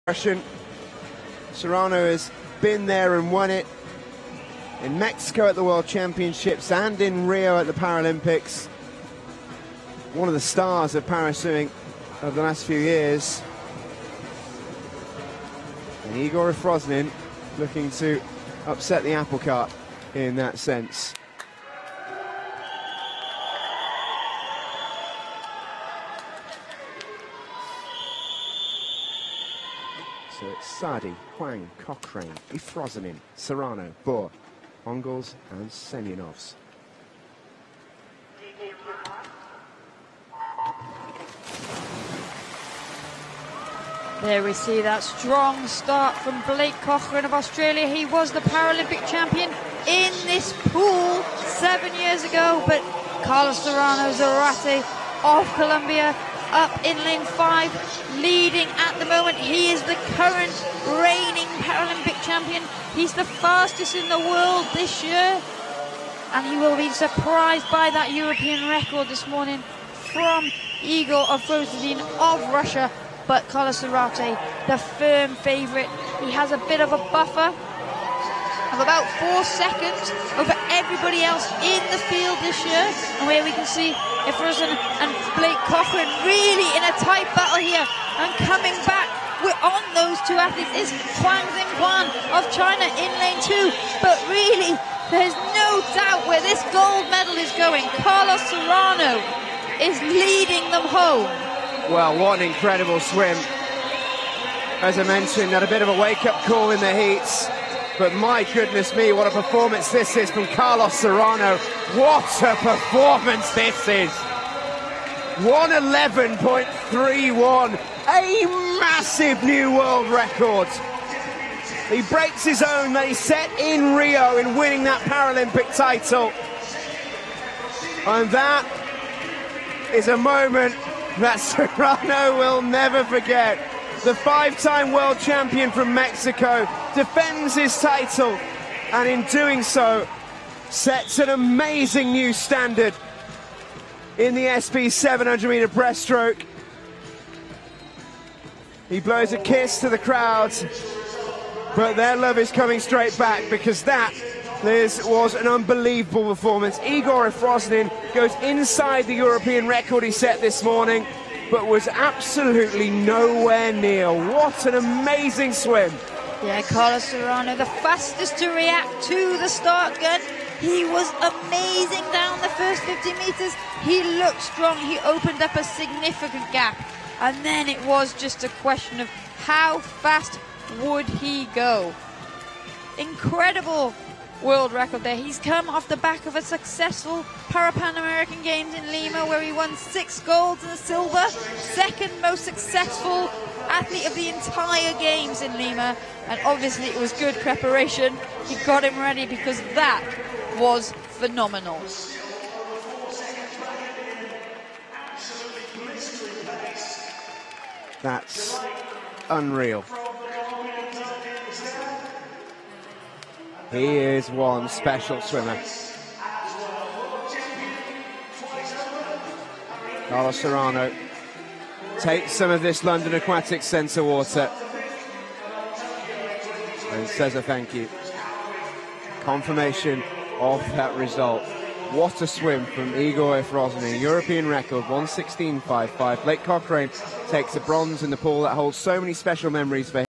Men's 100m Breastroke SB7 Final | London 2019
Final of the Men's 100m Breastroke SB7 on day three of the World Para Swimming Allianz Championships in London, 2019.

Results
Gold: Carlos SERRANO ZARATE (Colombia) (WR)
Silver: Egor EFROSININ (Russian Federation)
Bronze: Blake COCHRAINE (Australia)

More information and full results can be found on the World Para Swimming website.

Like and follow World Para Swimming on Facebook and Instagram at: @ParaSwimming and on Twitter at: @Para_Swimming